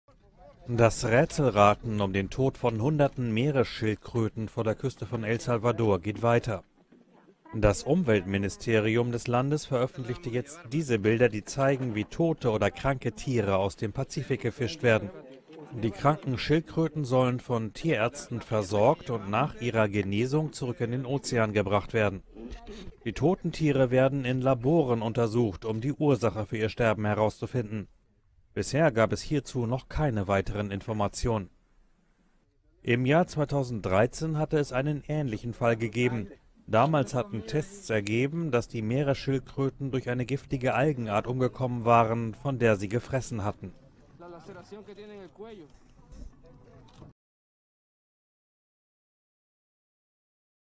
El Salvador: Schildkrötensterben in wirft Fragen auf
Noch immer ist unklar, warum Hunderte Tiere vor der Küste des Landes im Pazifik verendet sind.